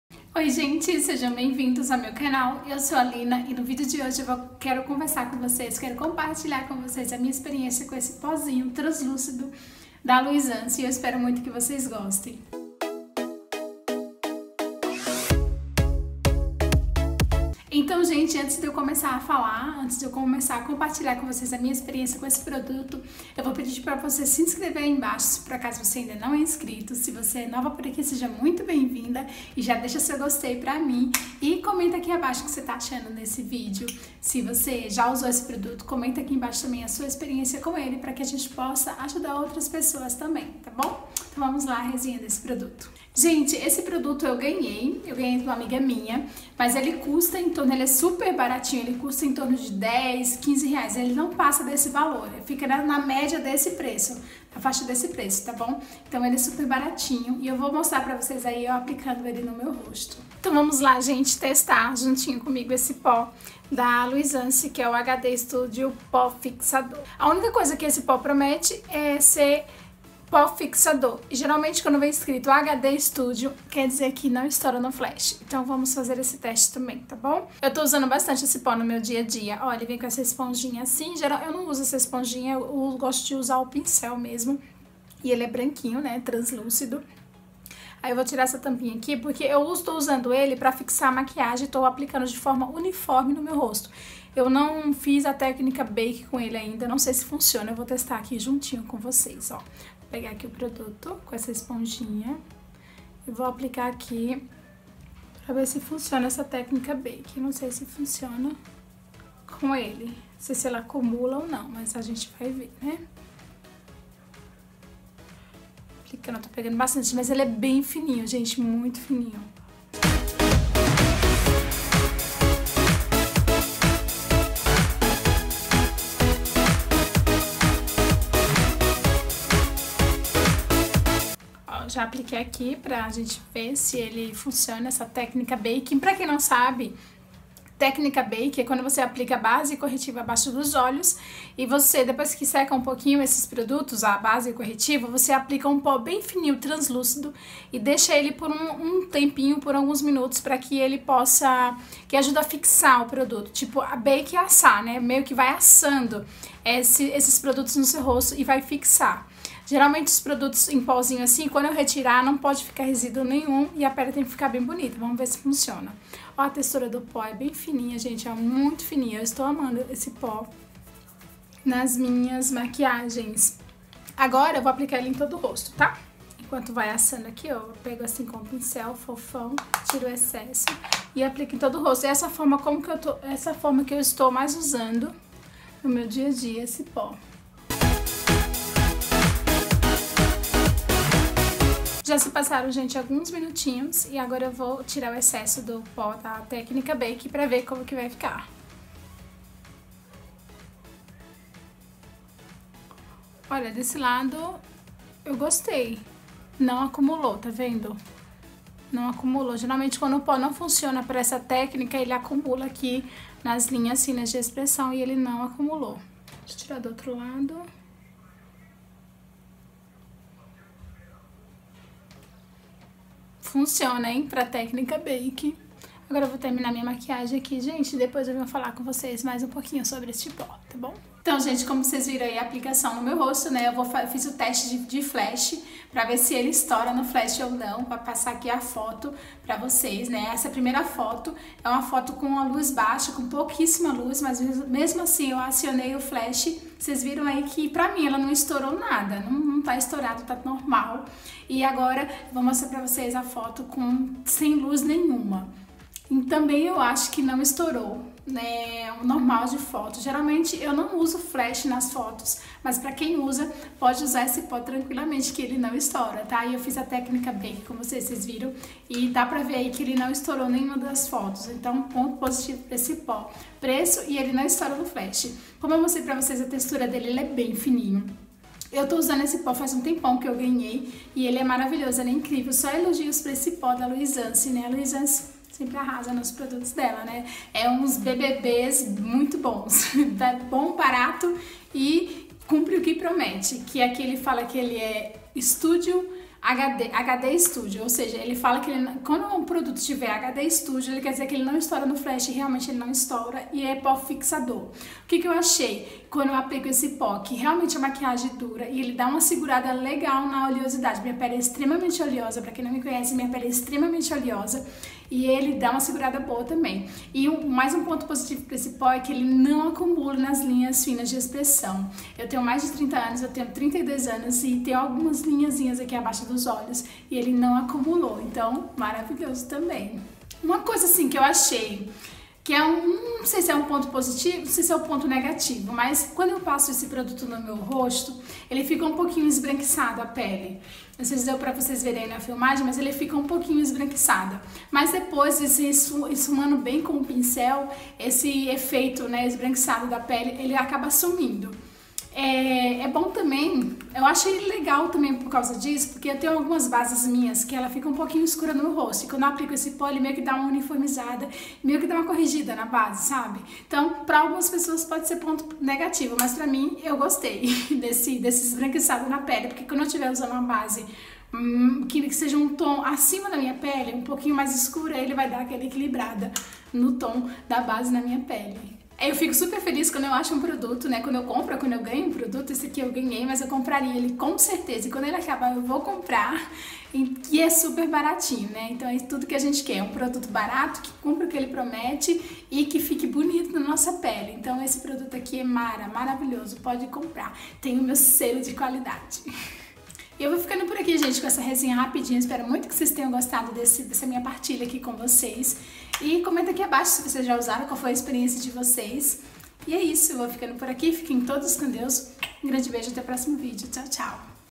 Pó Solto Translúcido Studio HD da Luisance | Makeup By Me | Lina Vaidosa
Oi, gente linda de meu Deus! Tudo bem com vocês? No vídeo de hoje eu compartilhei com vocês a resenha do pó HD Studio solto translúcido da Luisance. Espero muito que vocês gostem! Beijos!